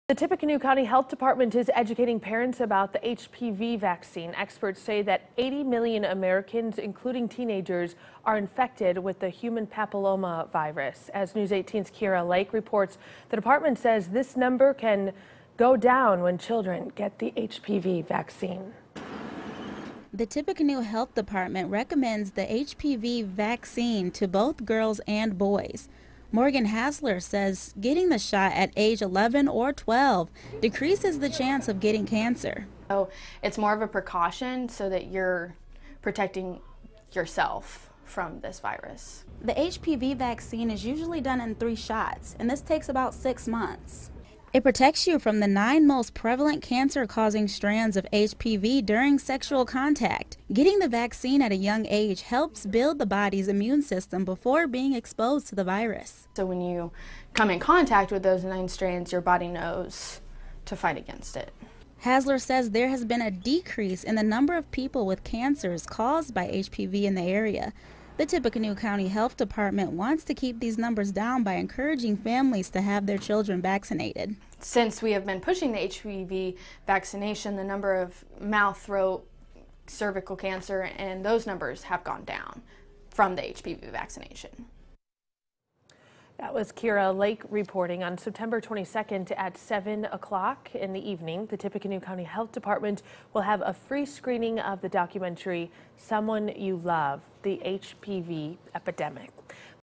Health Department: 1 vaccination can help protect children from cancer
TIPPECANOE COUNTY, Ind. (WLFI) —The Tippecanoe County Health Department wants parents to protect their children against cancer before it’s too late.